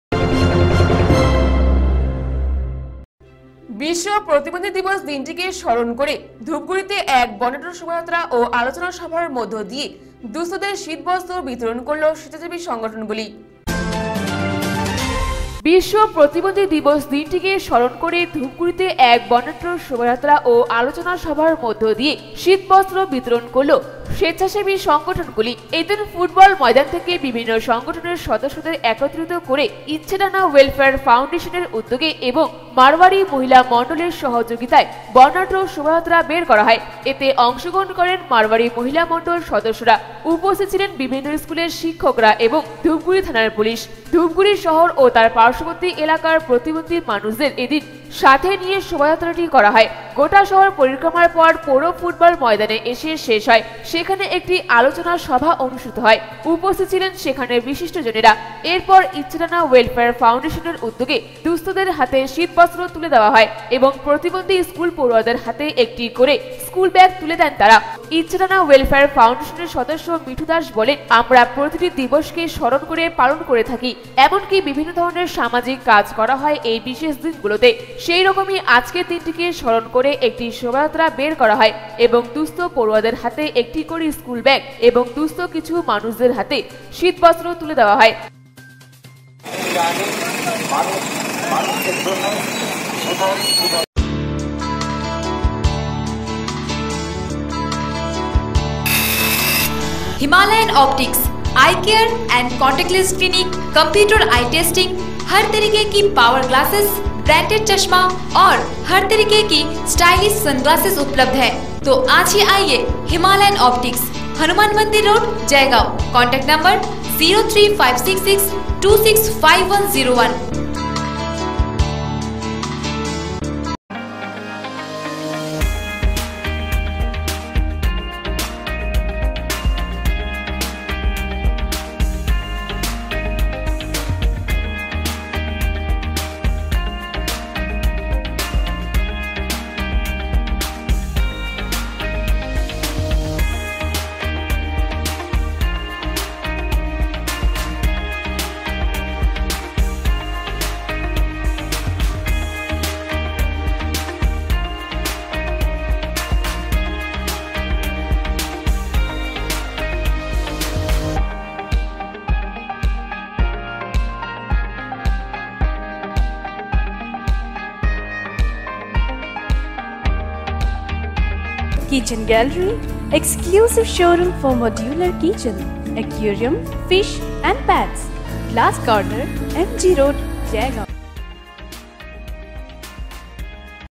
বিশ্ব প্রতিবন্ধী দিবস দিনটিকে স্মরণ করে ধূপগুড়িতেতে এক বর্ণাঢ্য শোভাযাত্রা ও আলোচনা সভার মধ্য দিয়
বিশ্ব প্রতিবন্ধী দিবস  দিনটিকে স্মরণ করে ধূপগুড়িতেতে এক বর্ণাঢ্য শোভাযাত্রা ও আলোচনা সভার মধ্য দিয়ে শীতবস্ত্র বিতরণ করলো স্বেচ্ছাসেবী সংগঠন গুলি